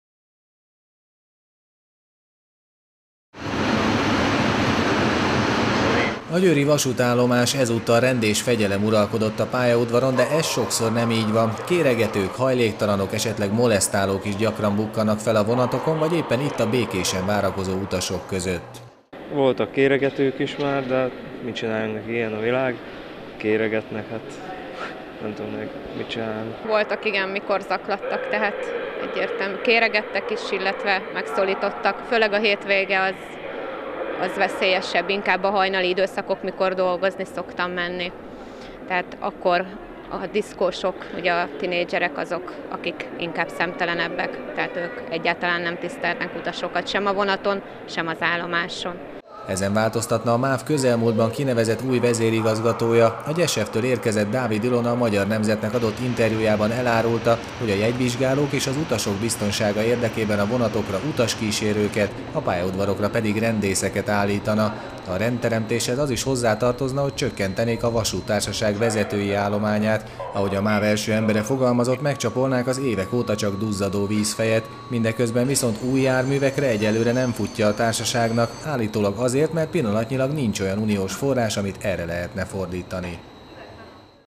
Rendet teremt
Komoly változtatásokat tervez a MÁV nemrég kinevezett vezérigazgatója, Dávid Ilona. A korábban a GYSEV - nél tevékenykedő vezető utaskísérő személyzetet tervez a jegyvizsgálók és az utasok biztonsága érdekében, a pályaudvarokon pedig rendészeket alkalmazna. A vezérigazgató szerint teljesen át kell alakítani a vasúttársaság szervezetét is, többek között a jelenlegi vezetőség egy részét egyszerűen elküldené a cégtől.